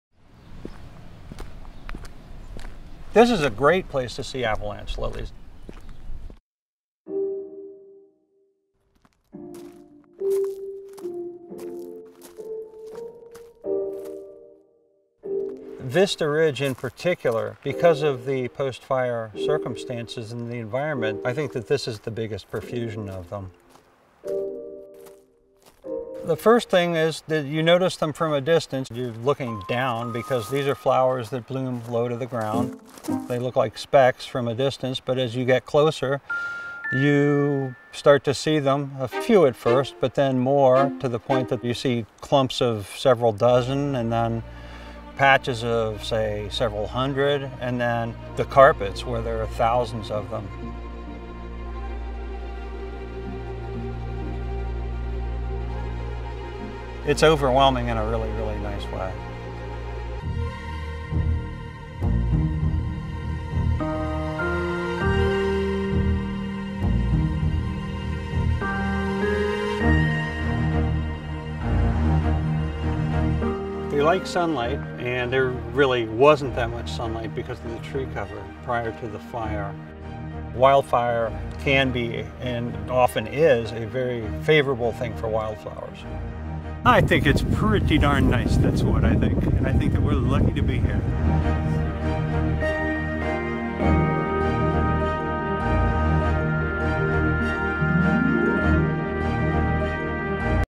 Avalanche Lilies Thrive After Fire
Avalanche Lilies explode on the North side of Mt. Hood in forests burned in the Dollar Lake fire.  The profusion of flowers burst forth in response to the abundant light.